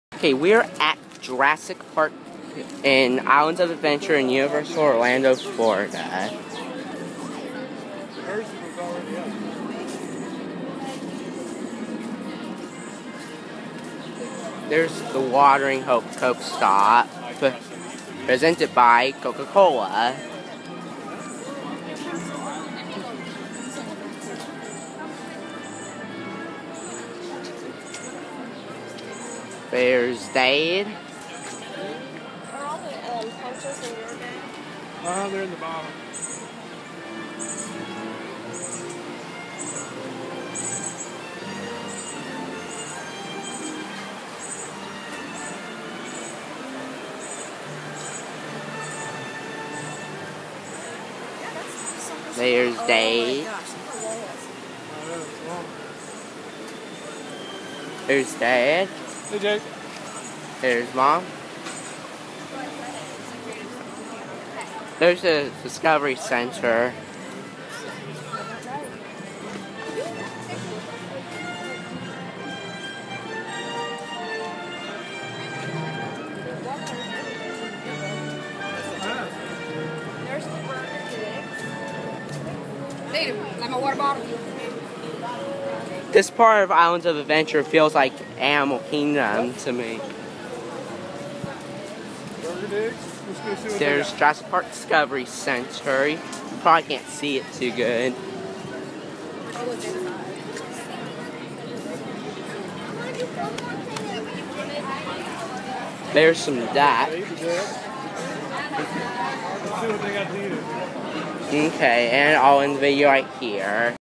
A Walkthrough Jurassic Park At Islands Of Adventure In Orlando, FL
We Went Through The Jurassic Park Section Of Islands Of Adventure In Universal Orlando, Florida And I Decided To Take A Quick Walking Video.

Location: Jurassic Park, Islands Of
Adventure
City: Orlando, FL